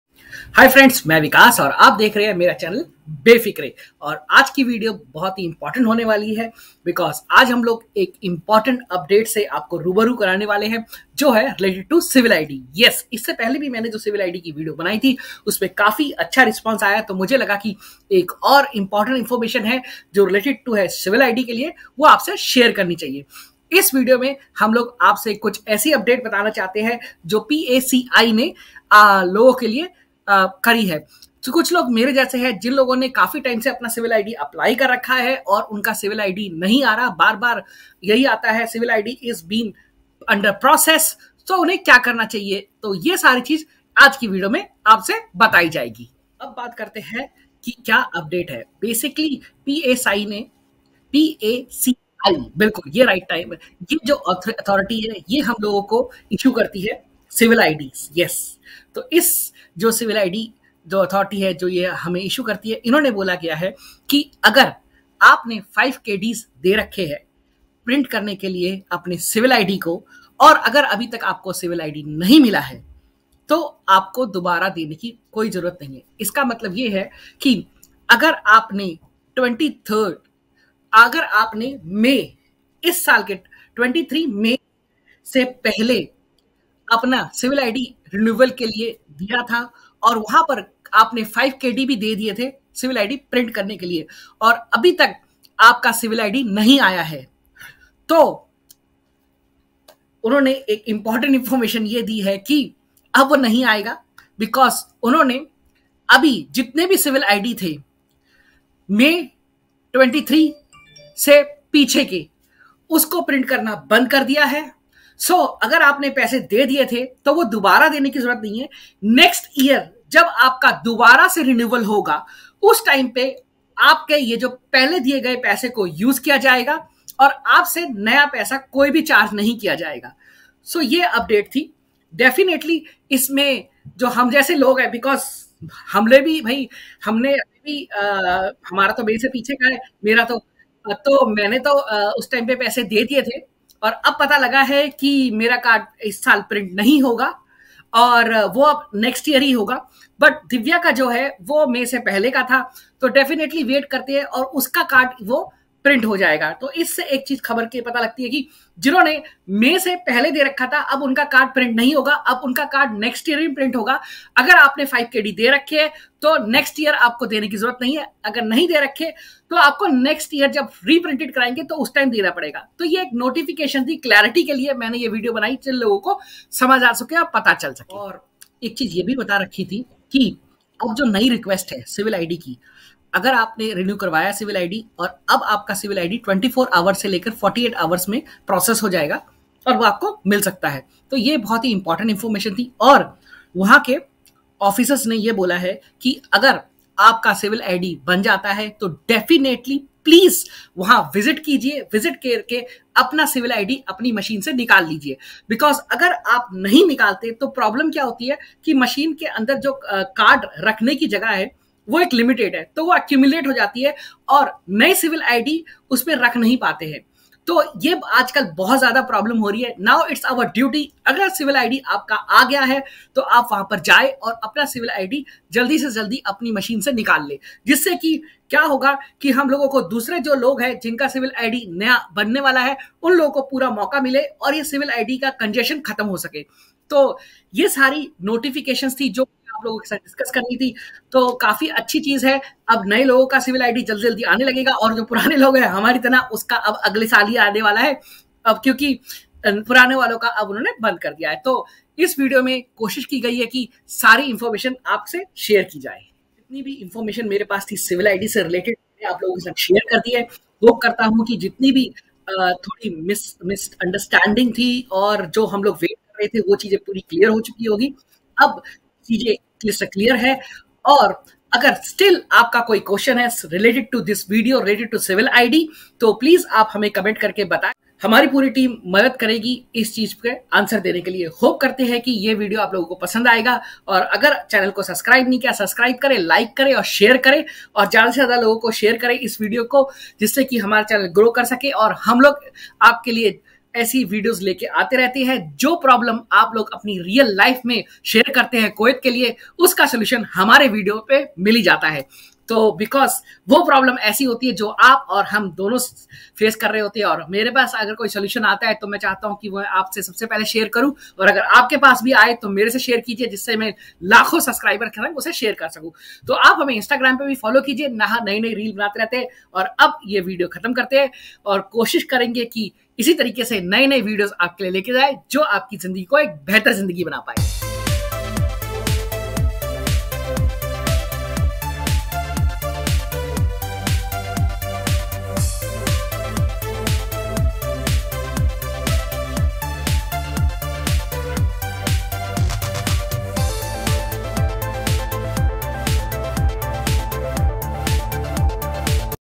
KUWAIT CIVIL ID UPDATE 2023 | ARE YOU WAITING FOR YOUR CIVIL ID FOR LONG TIME
PACI not to charge 5 KD card fee for those whose cards are not yet issued
Deputy Director General for Civil Registration Affairs at the Public Authority for Civil Information (PACI) Jaber Al-Kandari confirmed that PACI will not charge the card renewal fee of 5 KD for those whose cards are not yet issued during their next renewal.
The PACI stopped issuing cards for all those who submitted card renewal before last May 23. The new request are processed and cards are issued within 24 to 48 hours in most cases.
As the old requests are not processing, those who paid the 5 KD fee earlier will no need be required to pay the fee again during their next renewal request, Jaber Al-Kandari said
He also urged all residents to collect the cards once it is ready so that they do not accumulate in the machines, which delays the issuance of new cards